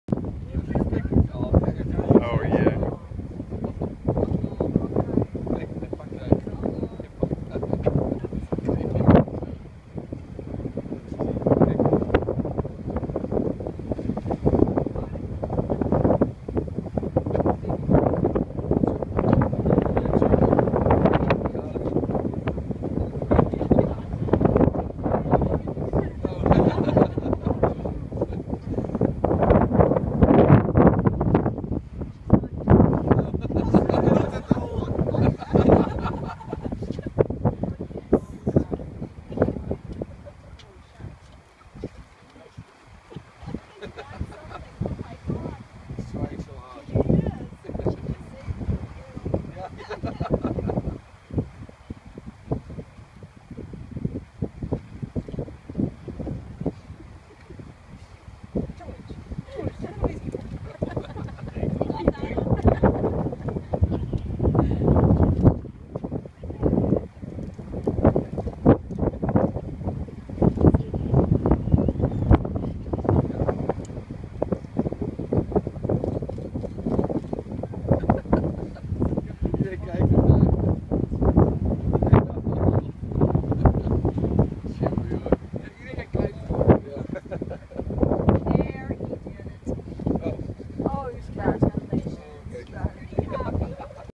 African (Jackass) Penguin Love in South Africa
Penguins need love too!

At Boulders Beach in Cape Town, South Africa